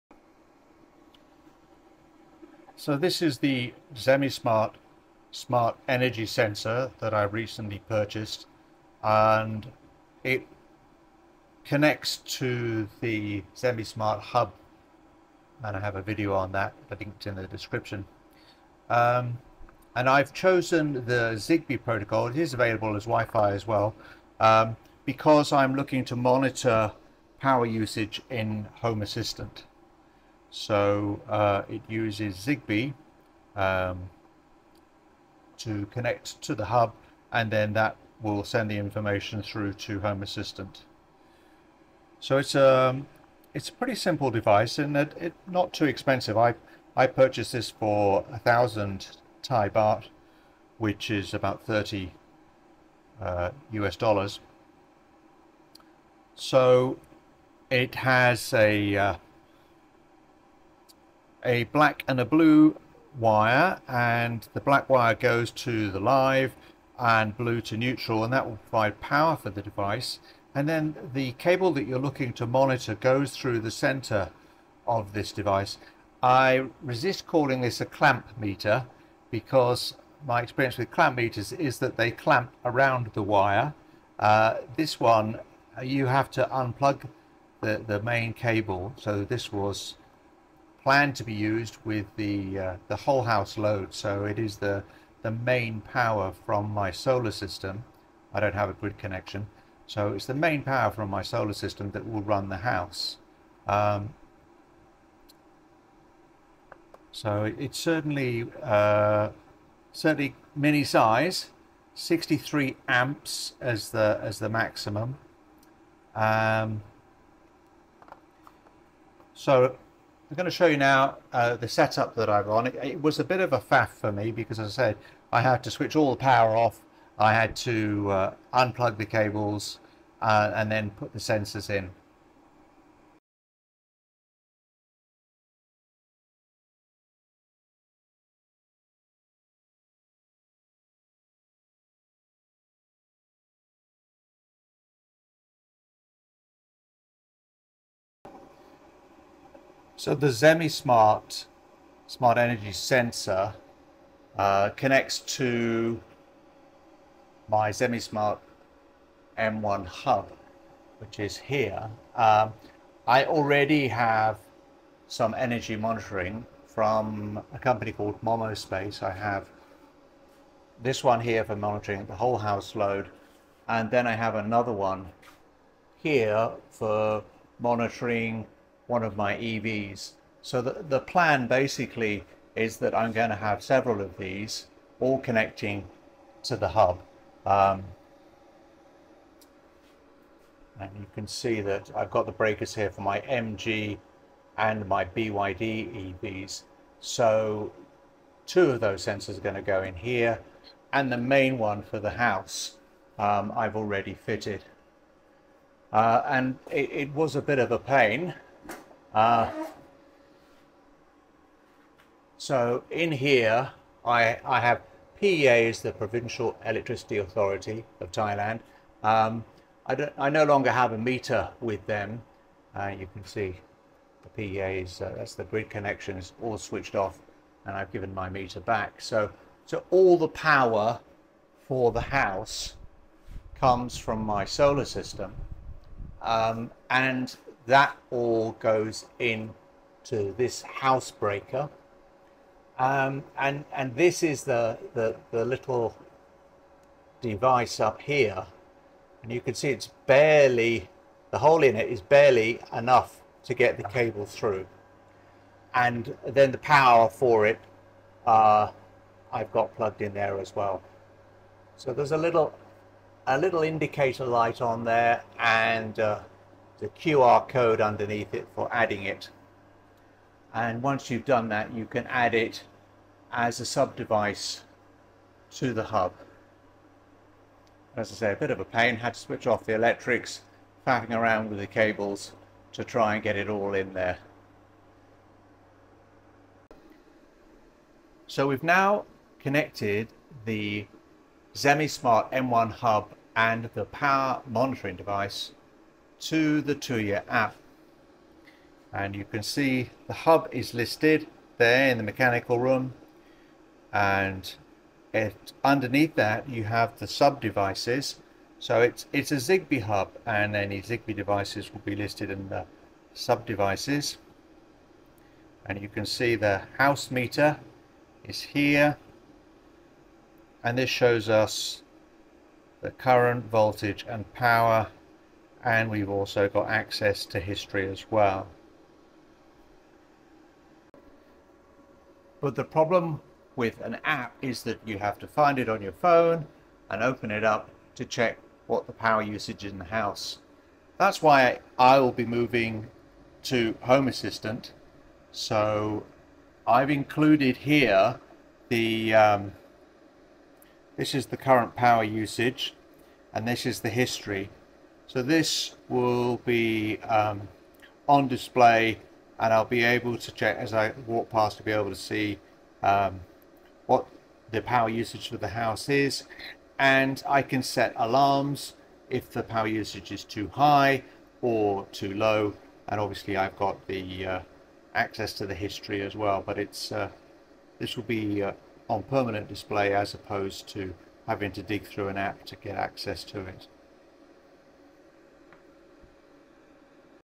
Zemismart SPM01 Smart Energy Monitor
To be able to use Tuya Smart Energy Monitors with Home Assistant they need to be Zigbee and connect to a Zigbee hub like

I did an update in the Tuya app and the device then failed to provide accurate power data. I had to remove it from the app and then do a factory reset by holding down the reset button then add it back into the Tuya app